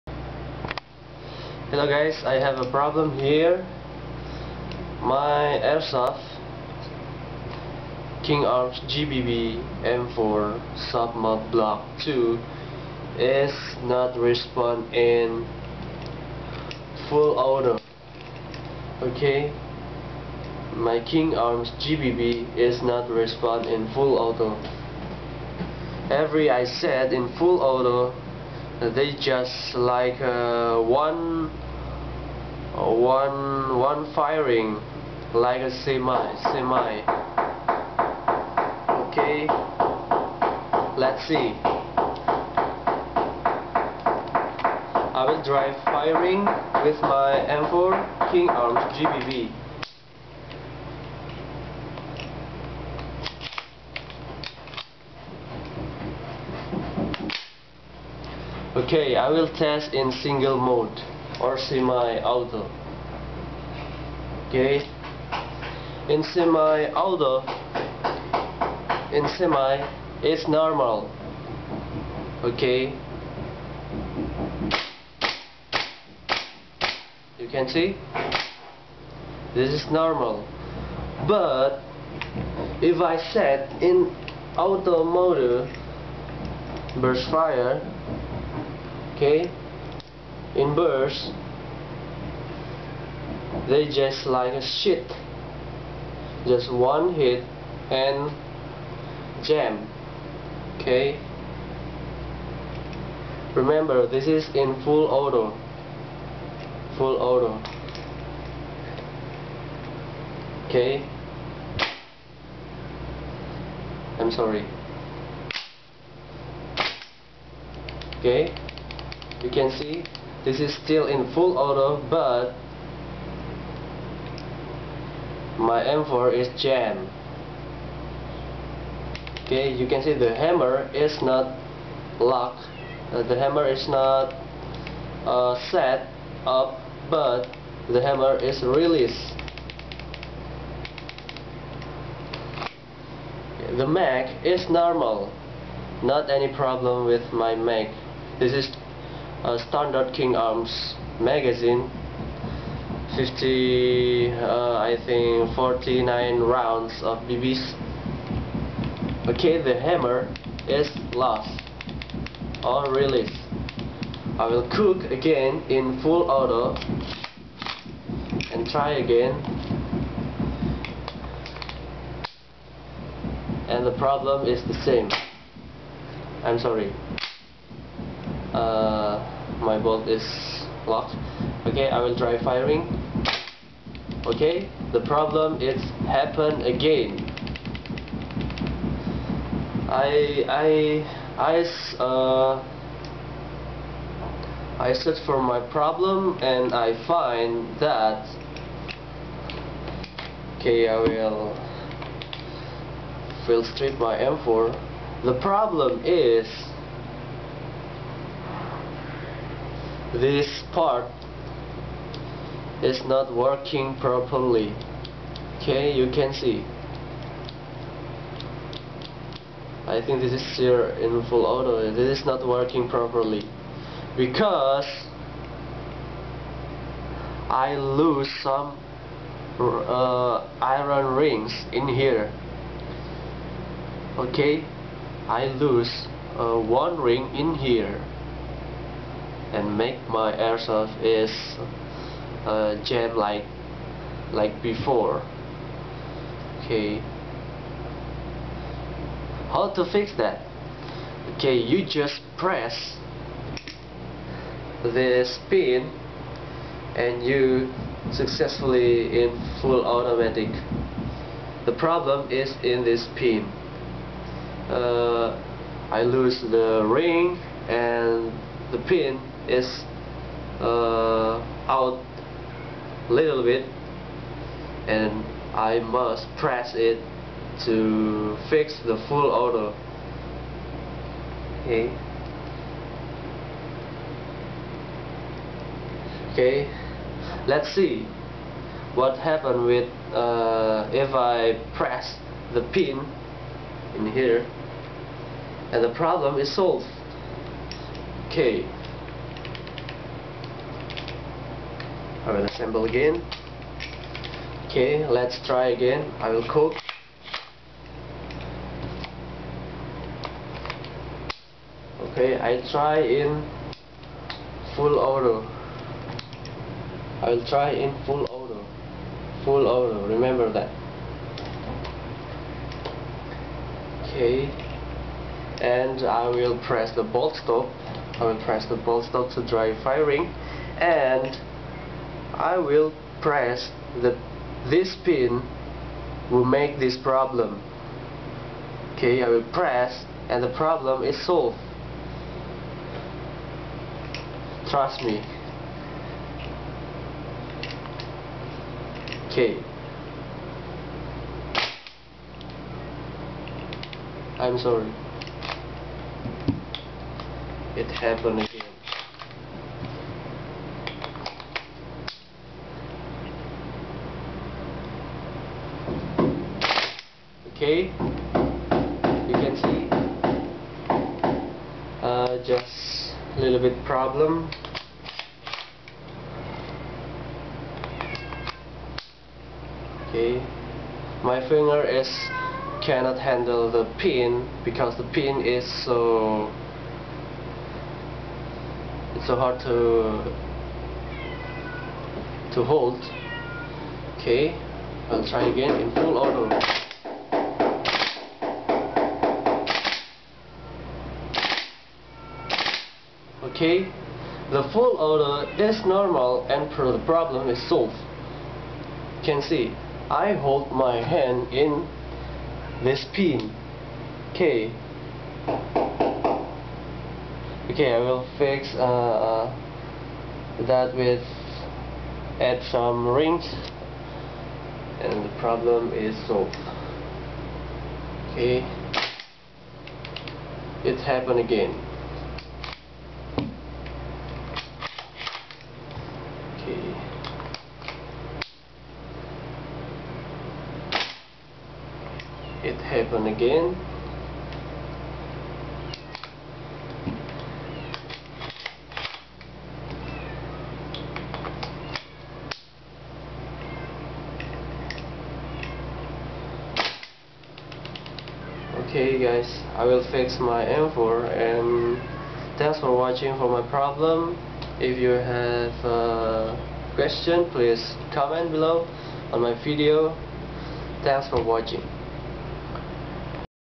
King Arm's GBBR M4 Problem with Full Auto and How to Fix it!! PART 1
Hello guys, this is my problem with my M4 GBBR King Arms. The problem is, i cant firing in full auto. But i can firing fine in semi. Who makes this problem? I lose 1 ring to hold the auto sear pin, this make full auto not working properly. To fix that, you just hold the pin with your thumb, and try to firing in full auto. voila the problem is solved, but you cant handheld the pin for long time, becoz the pin is so hard to come out when firing in full auto. You must search a new ring, to solve the problem. Thanks uys for watch. I hope this video will help you. bye